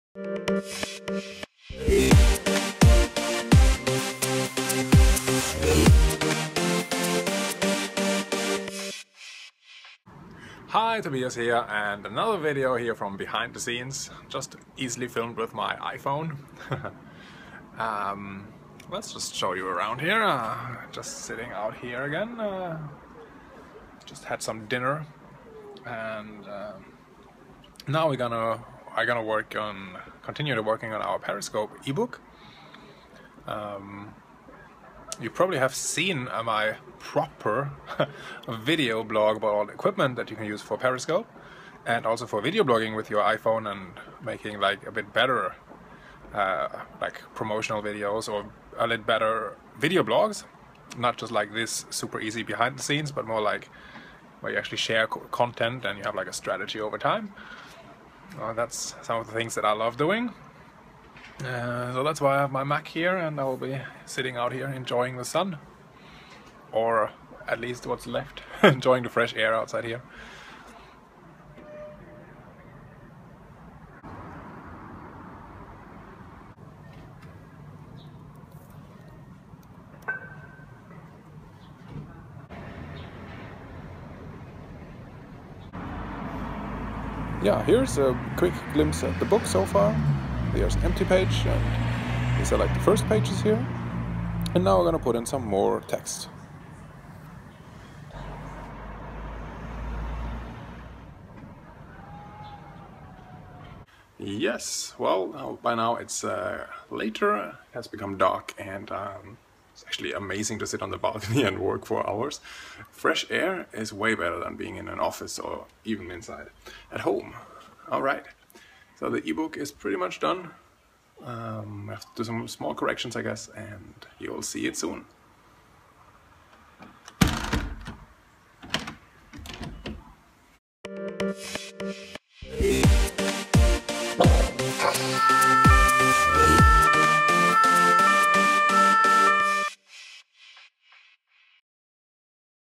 Vlog 3: eBook, design, Periscope
- Vlog3: How I design an ebook for Vanessa Raphael... it is finished now.